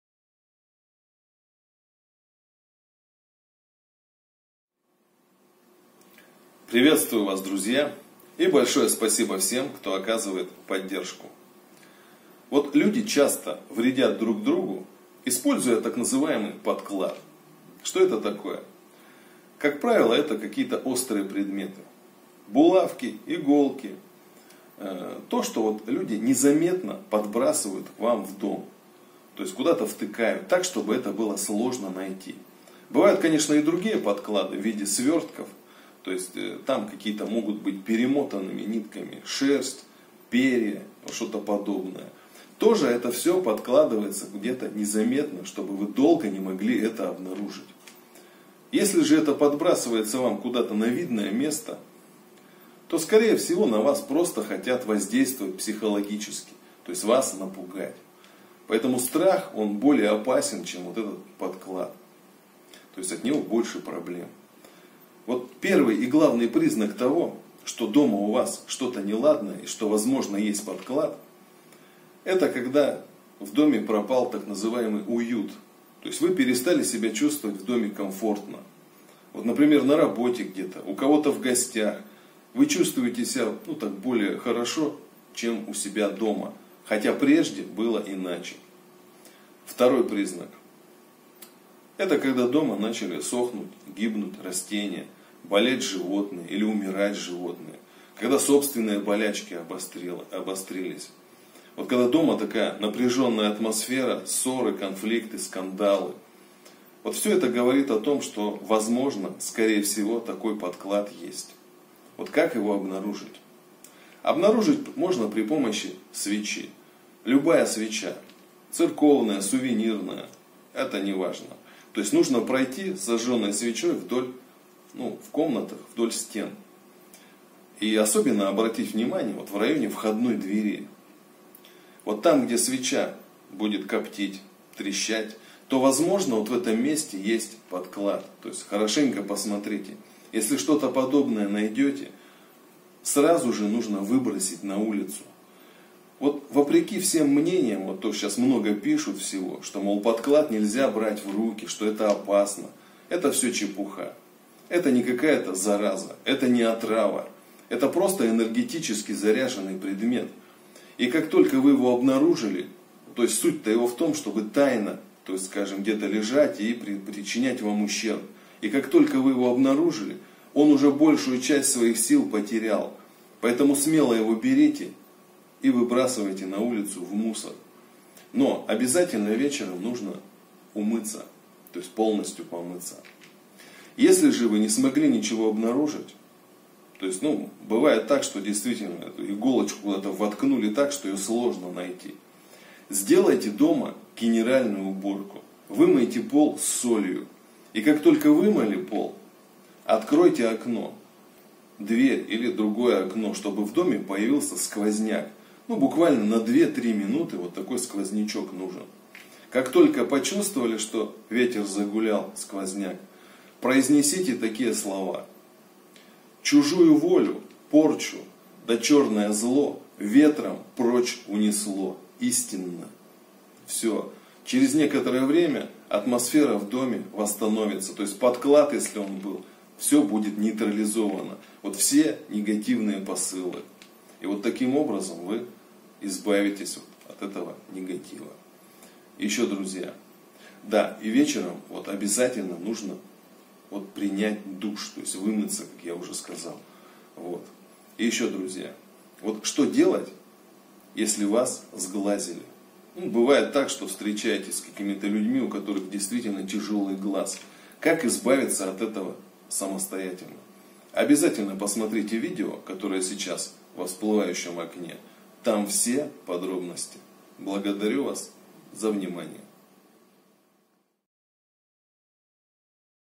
Выбросьте эту вещь и всё зло вернётся врагам. Как нейтрализовать подклад
Как найти подклад в доме. Мастер Владислав рассказывает о том, что первый признак того, что дома есть подклад – это пропал домашний уют. Второй признак – это напряженная атмосфера, сохнут растения, болеют животные, ссоры и ругань в доме. Если вы не нашли подклад, сделайте дома генеральную уборку, вымойте пол с солью, а затем на несколько минут откройте окна, чтобы был сквозняк и когда это почувствуете, произнесите такие слова:
«Чужую волю, порчу, да чёрное зло, ветром прочь унесло. Истинно».
И подклад будет нейтрализован

Яндекс счёт (ю- Моней) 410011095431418